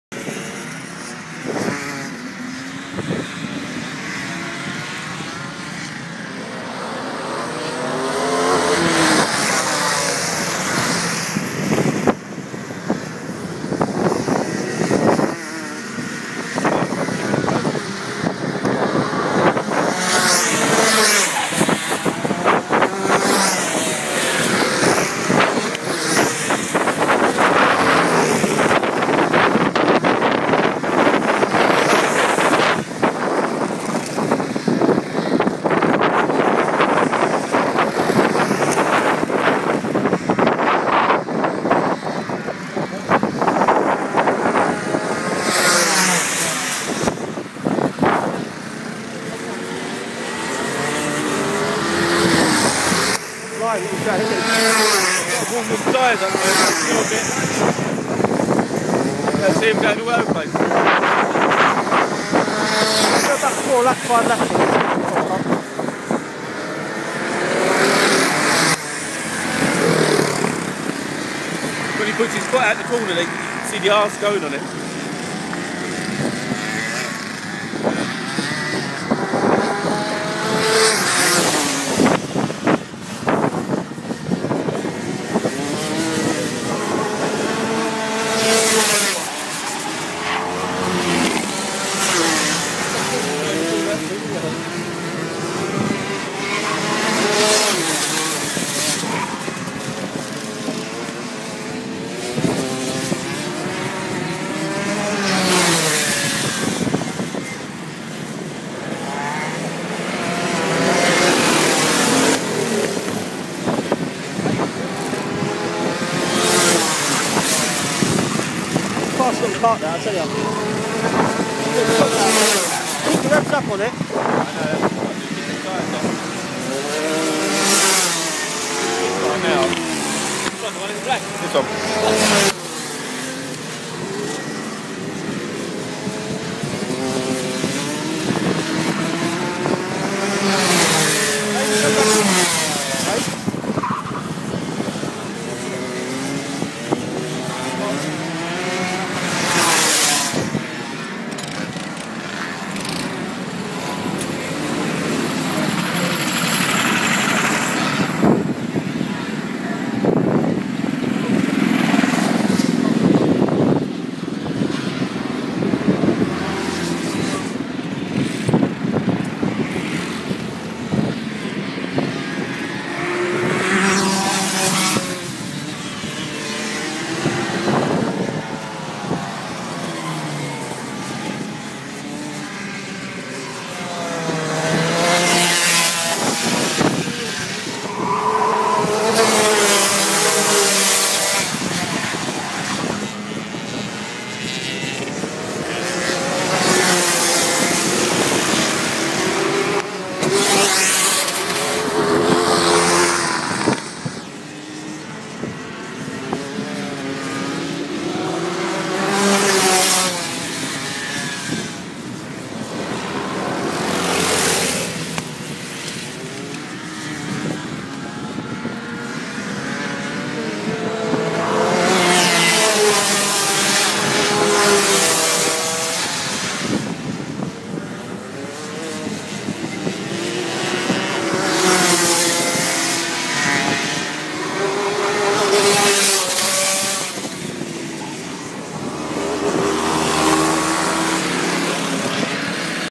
Tilbury karting
Biland, rotax and pro karts driving around tilbury. my first ever go in the biland